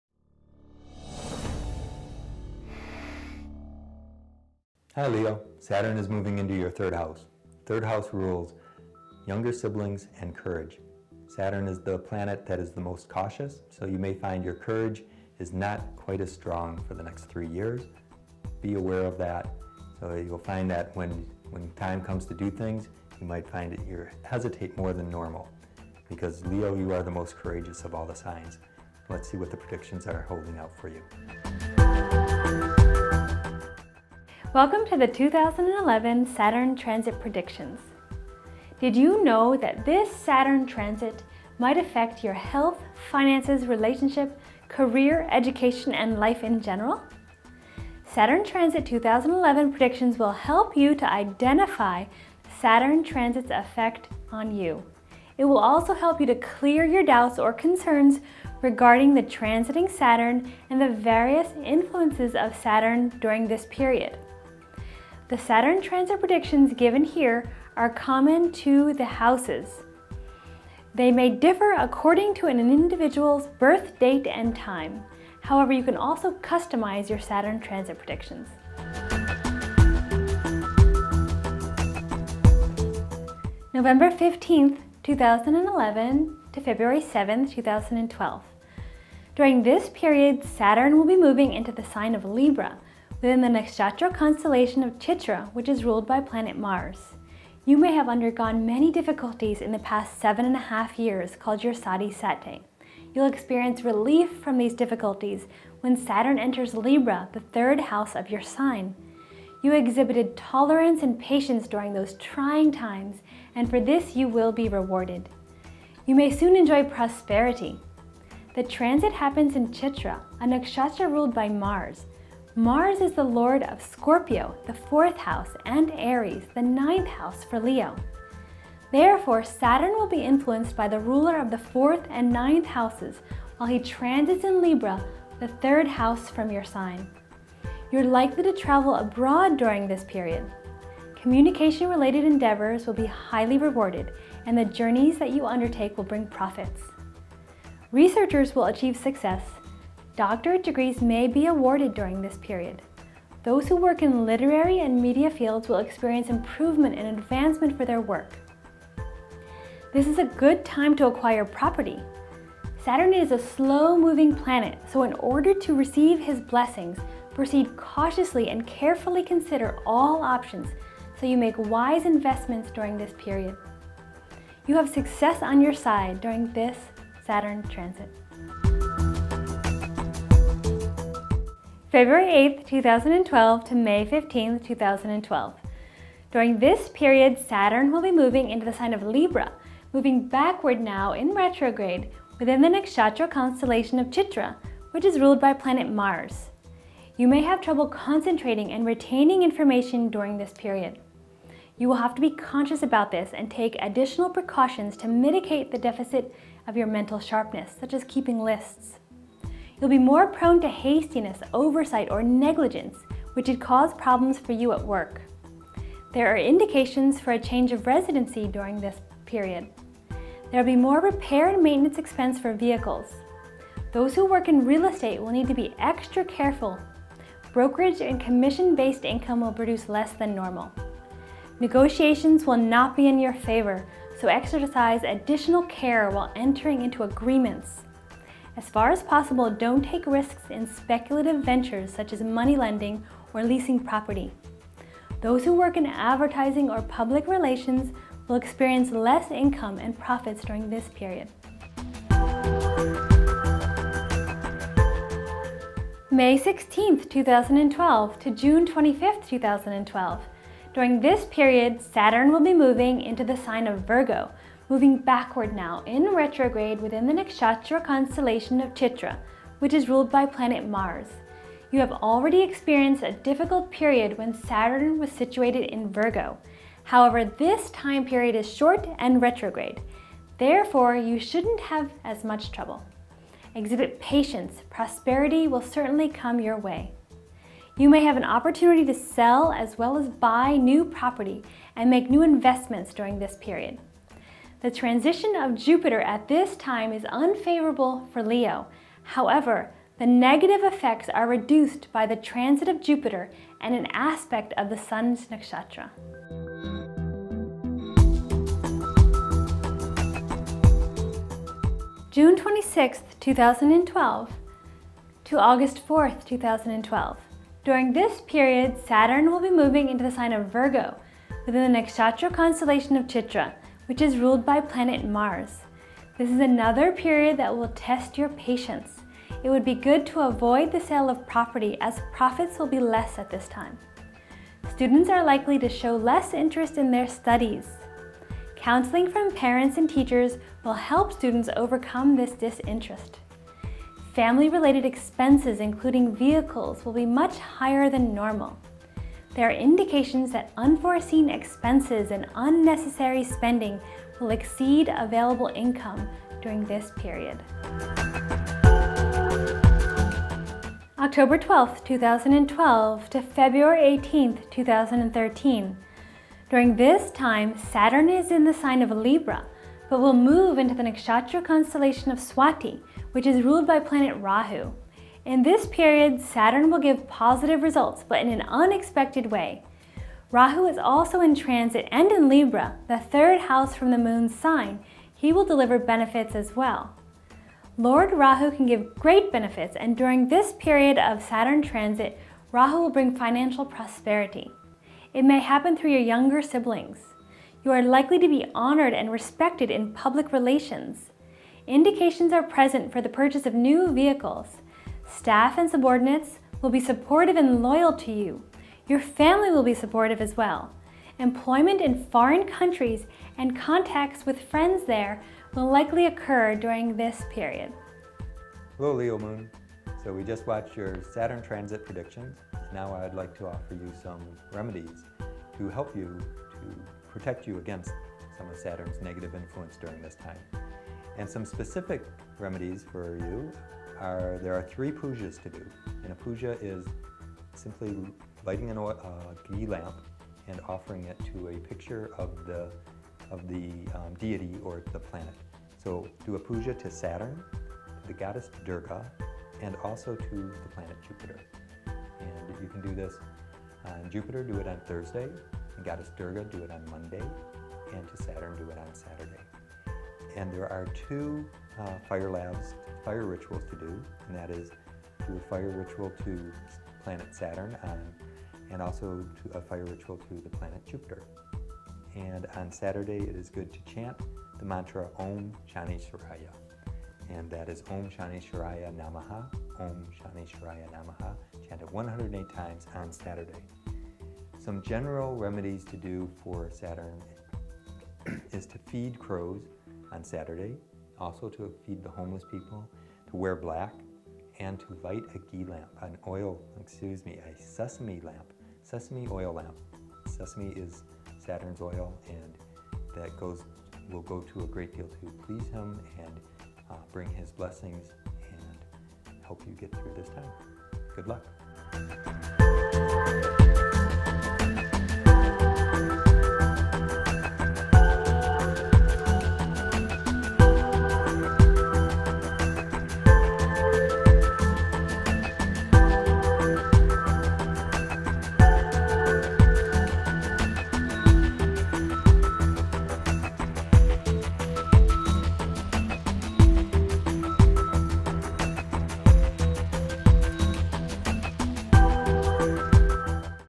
Saturn Transit into Libra -- Leo Moon Sign Predictions
Leos -- Do you know that this Saturn Transit (15th Nov to 2nd Nov 2014) might affect your Health, Finance, Relationships, Career, Education, Life etc?
Saturn Transit 2011 Predictions will help you to identify the Saturn Transit Effects on Leos. It will also help you to clear your doubts or concerns regarding the transiting Saturn and the various influences of Saturn during Saturn Transit 2011.

The Saturn Transit Predictions given here are common for the natives of Leo Moon sign and may differ according to an individual's birth date and time; however, you can also customize your Saturn Transit Predictions.

November 15th 2011 to February 7th 2012:
During this period, Saturn will be moving into the sign of Libra, within the Nakshatra Constellation of Chitra which is ruled by the planet Mars.
You may have under gone many difficulties during the past seven and half year Saturn period ("Sade Sati"). You will have relief from these difficulties when Saturn enters Libra, the 3rd house of your sign. You exhibited tolerance and patience during those trying times and for this, you will be rewarded. You may soon enjoy prosperity. The transit happens in Chitra, a nakshatra ruled by Mars. Mars is the Lord of Scorpio, the 4th house and Aries, the 9th house for Leo. Therefore, Saturn will be influenced by the ruler of the 4th and 9th house while he transits in Libra, the 3rd house from your sign.  You are likely to travel abroad during this period of Saturn Transit. You will benefit much from communication related endeavors. The journeys that you undertake will bring profits. Researchers will achieve success. Doctorate degrees may be awarded during this period. Those who work in literary and media fields will experience improvement and advancement in their work during this period of Saturn Transit.
This is a good time to acquire property. Saturn is a slow moving planet, so to receive his blessings, proceed cautiously and carefully consider all options so that you make a wise investment during this period.  You will have success on your side during this period of Saturn Transit.
To know more on Saturn Transit Libra -- Leo Moon sign predictions.